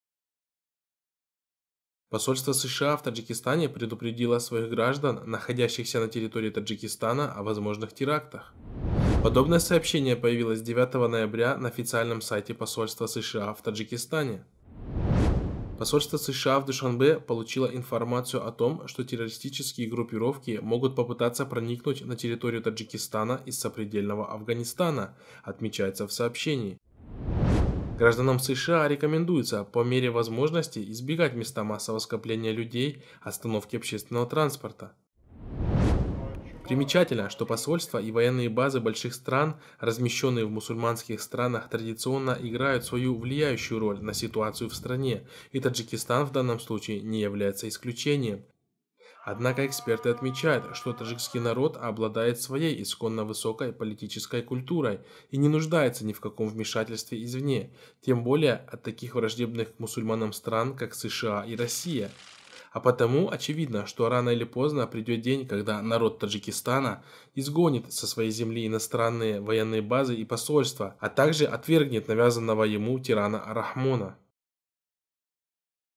Посольство США заявило об угрозе терактов в Таджикистане
Посольство США в Таджикистане предупредило своих граждан, находящихся на территории Таджикистана, о возможных терактах.
Подобное сообщение  появилось 9 ноября на официальном сайте посольства США в Твджикистане.